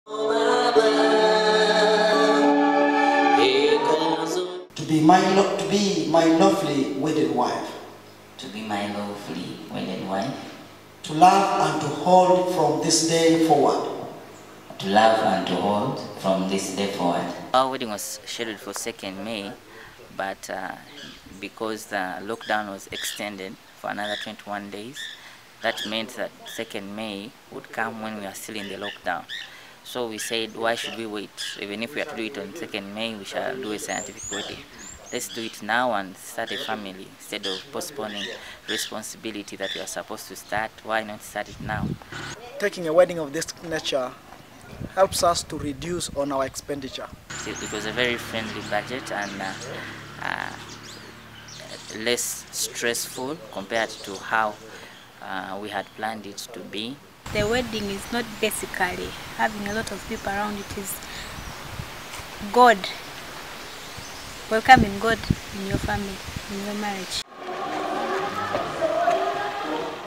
Kumi couples opt for scientific weddings
As the lockdown continues across the country, many married couples in Kumi have opted for “scientific” weddings as one of the ways to strengthen their marriages but also trim down their wedding budgets.

Bishop Simon Peter Eliodo said that many couples have adjusted downwards their wedding dates to fit within the lockdown.